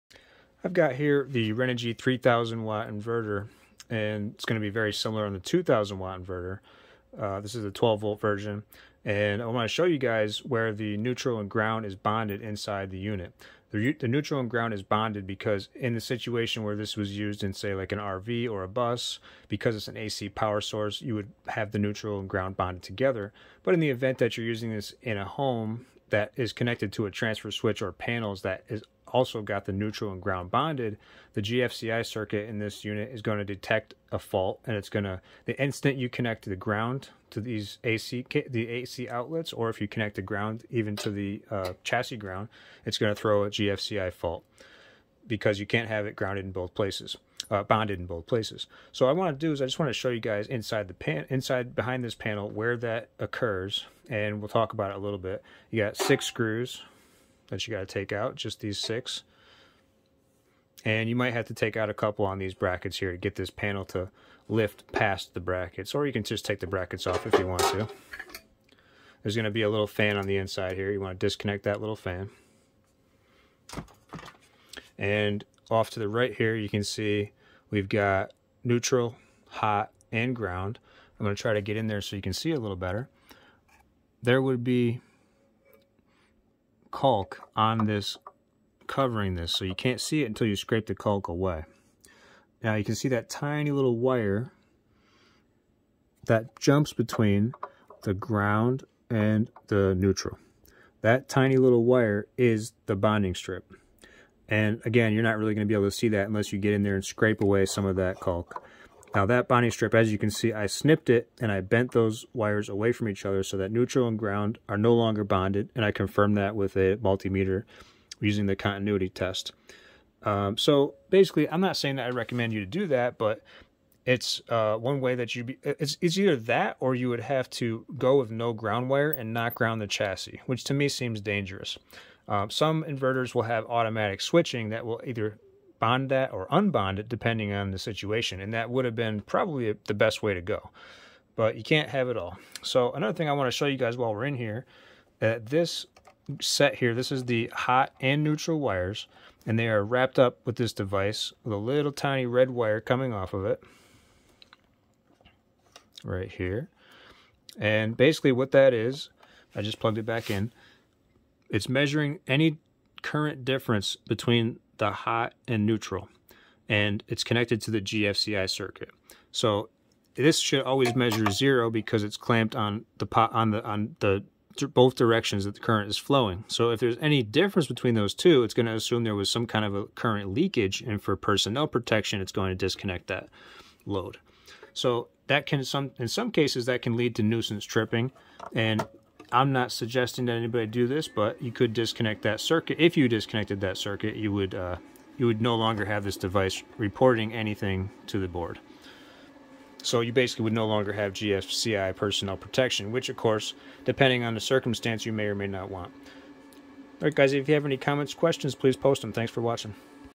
Unbonding the Ground and Neutral on Renogy Inverter
This video will help you locate where the neutral is bonded to the ground inside Renogy Pure Sine Wave Inverters.   I will also locate the GFCI sensor.